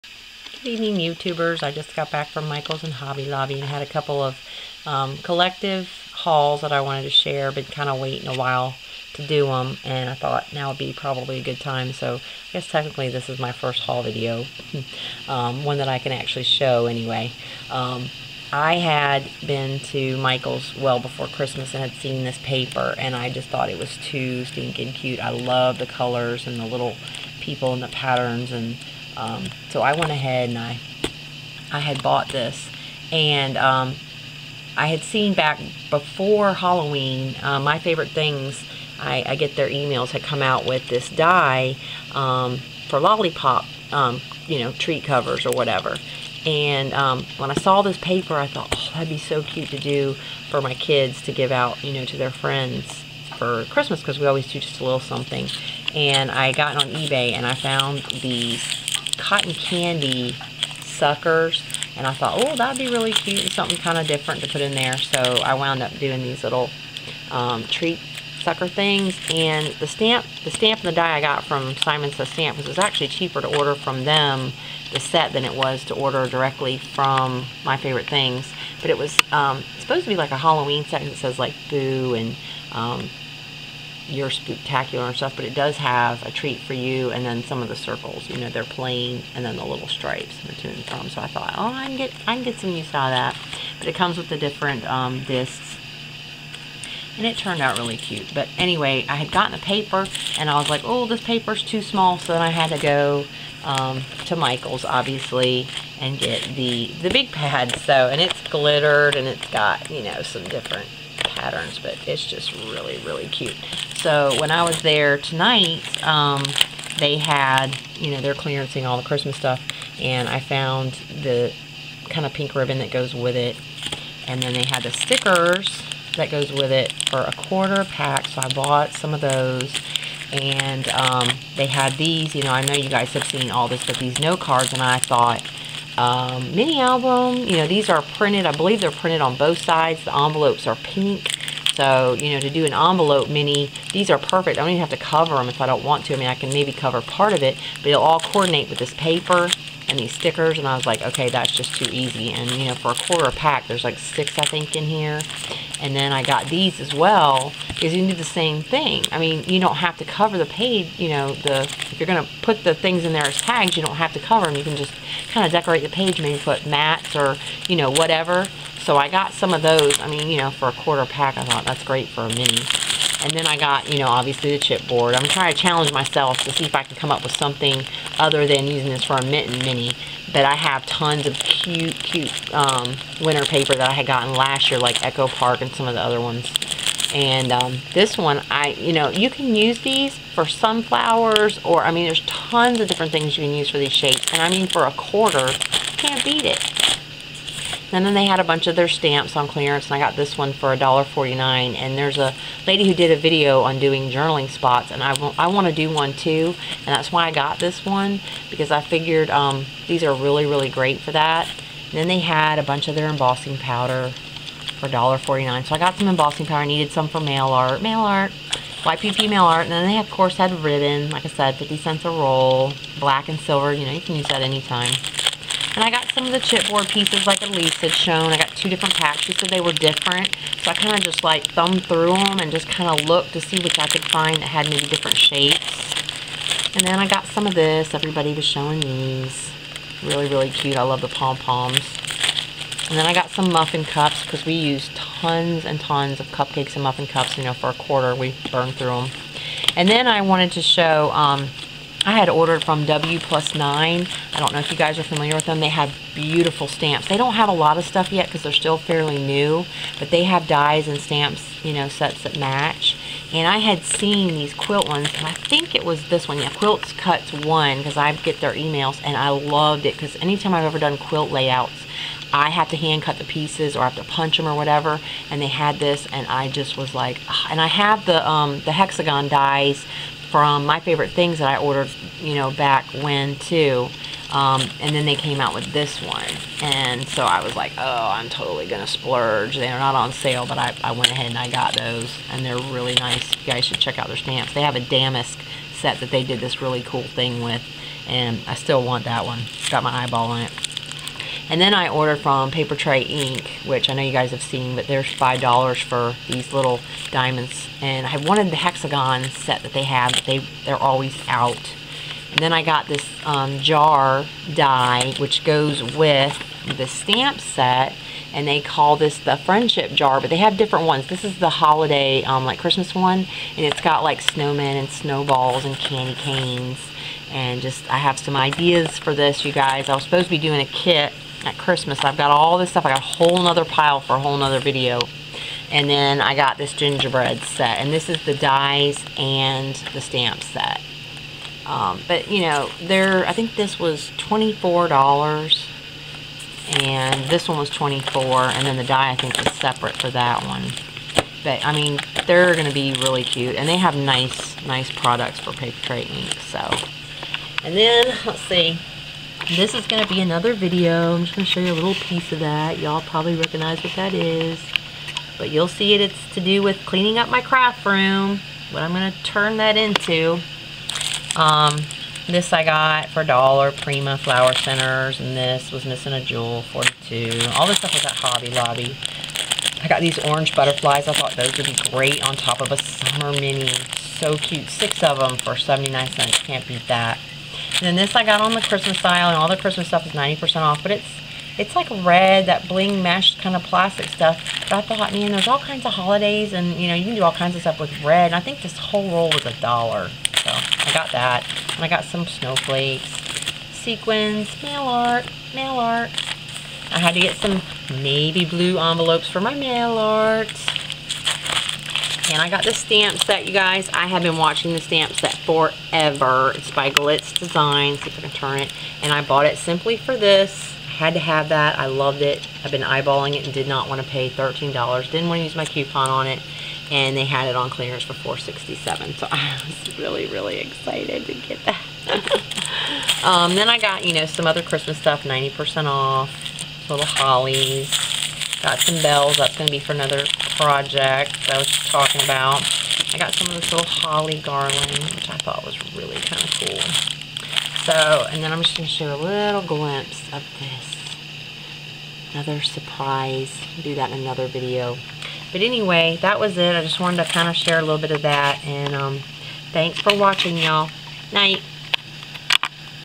1st Haul Video papertrey Ink (PTI) WPLUS9 My Favorite Things (MFT) Mike's Ho Lo & Home Depot?! Jan13
Hey y'all! This is a collective haul vid, my hubs knows about all this stuff :) so i can show my utube friends, lol! so sorry, its shaky i filmed with one hand and I was going really fast, so i didnt run out of memory card. If you have any questions, ask away! As always...thanks for being there to watch :)  plus 2 tiny secret reveals for upcoming videos!